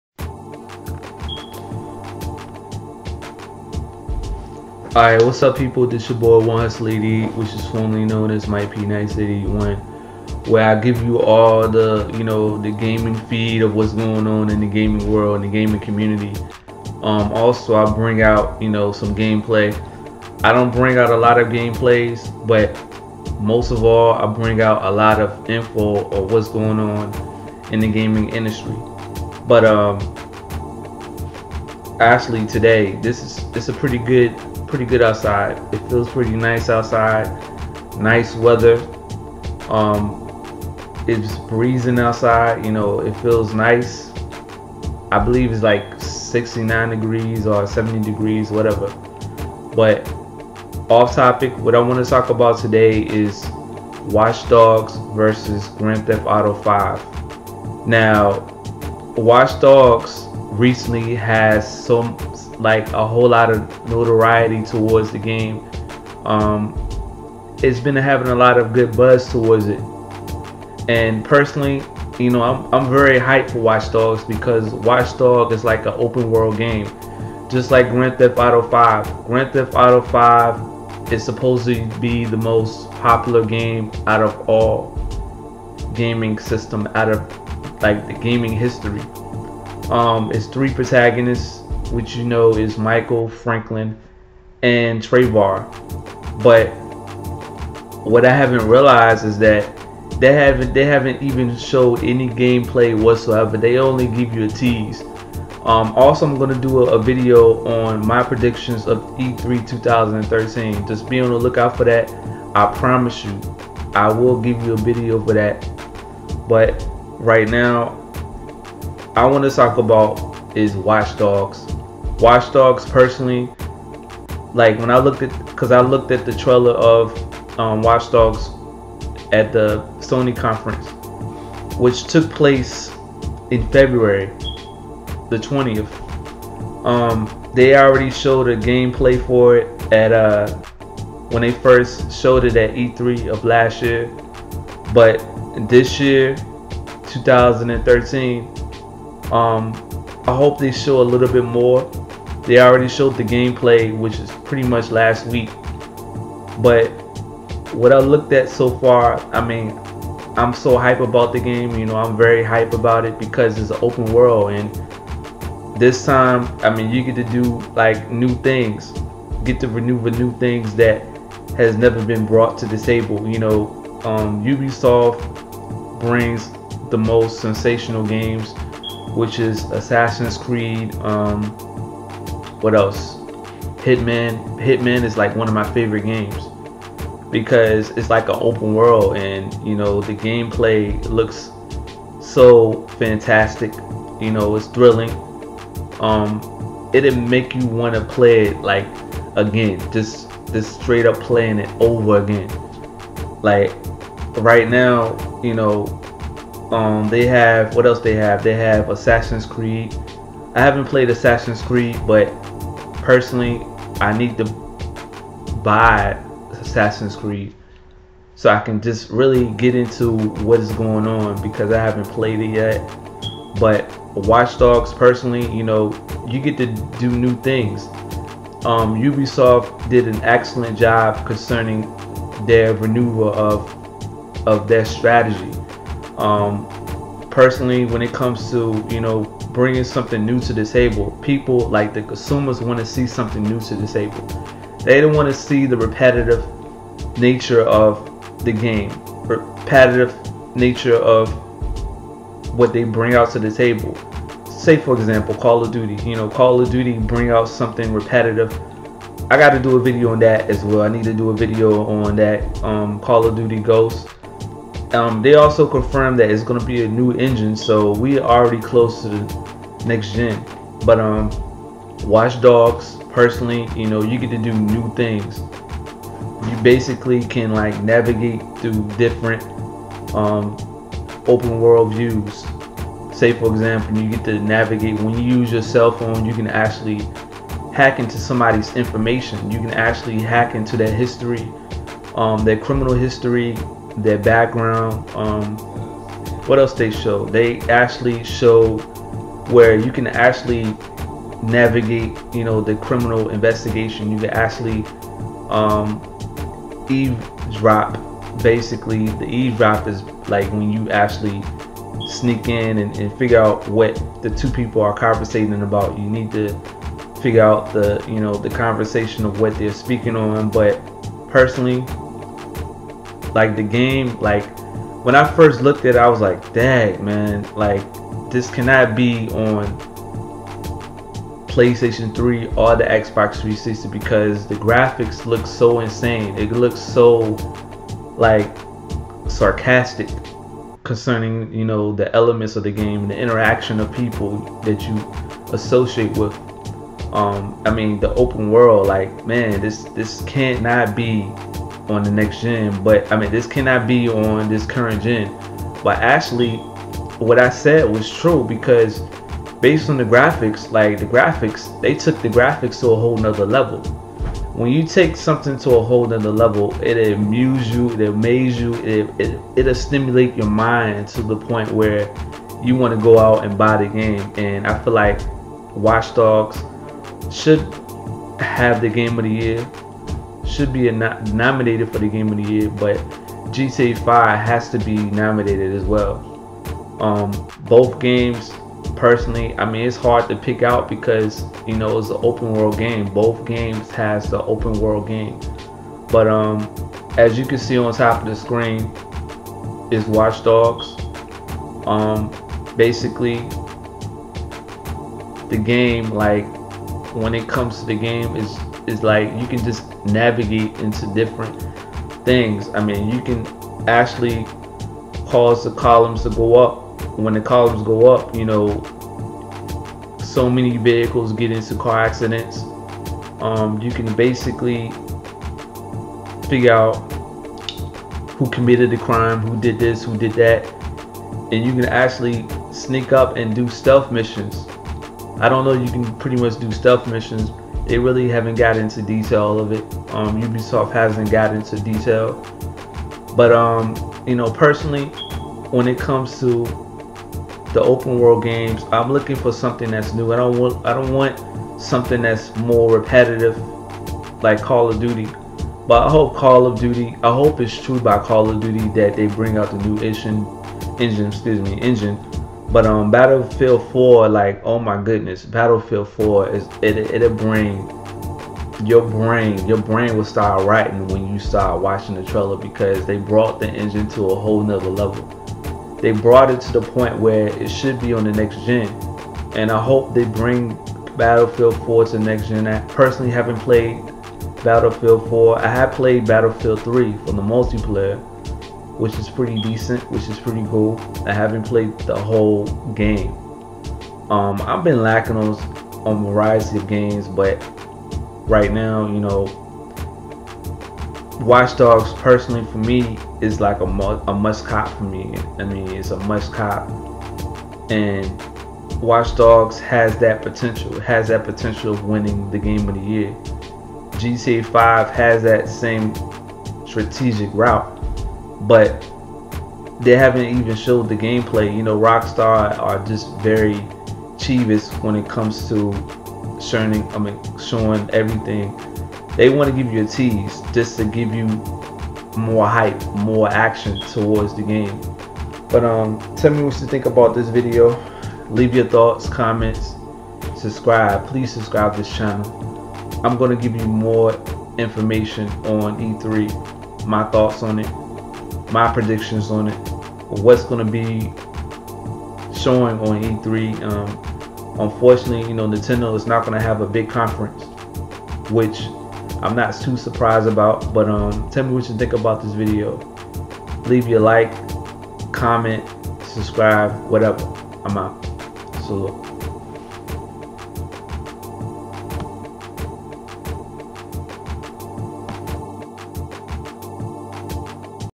Watch Dogs Vs GTA V: The Game of the year
This video is my impressions and thoughts on Watch Dogs; which is one of my top 5 anticipated games of all times. More videos coming soon. Don't forget to check out my video on my predictions of E3 2013 which is coming this week or next week.